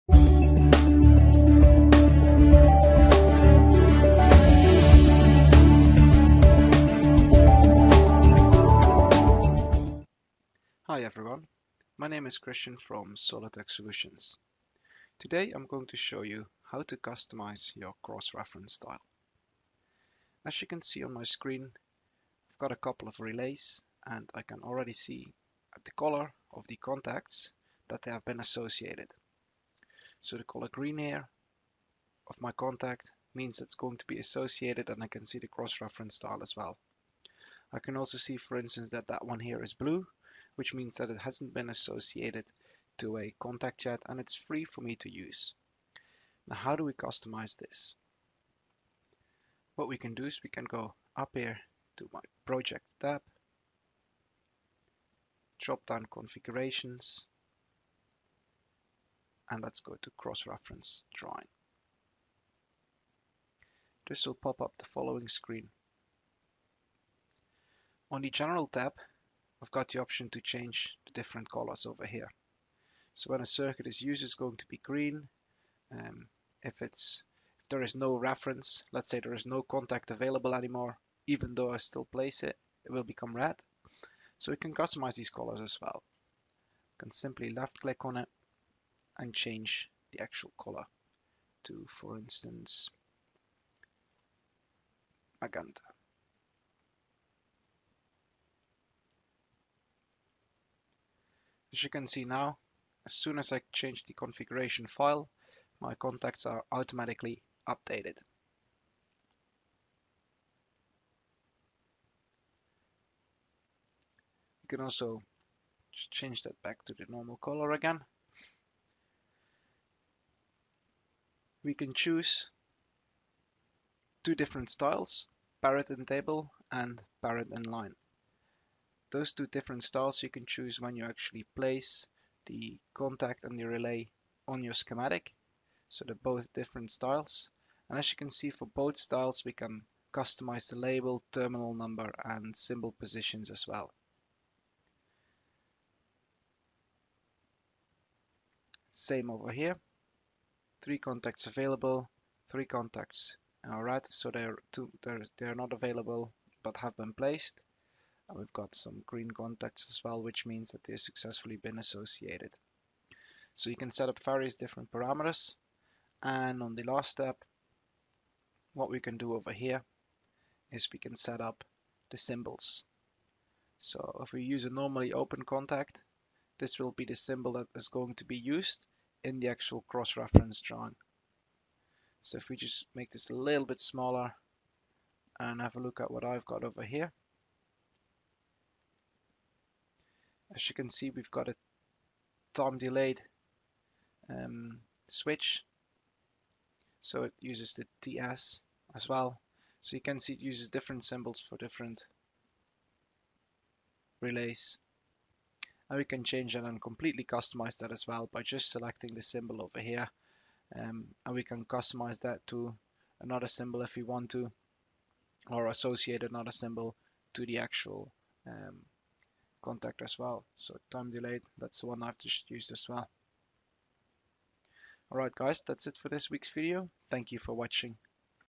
28. SolidWorks Electrical - How to Customise the Cross-Reference Contacts of a Relay
This video demonstrates how to customise the cross reference contacts of a component in SolidWorks Electrical. By Christian van der Horst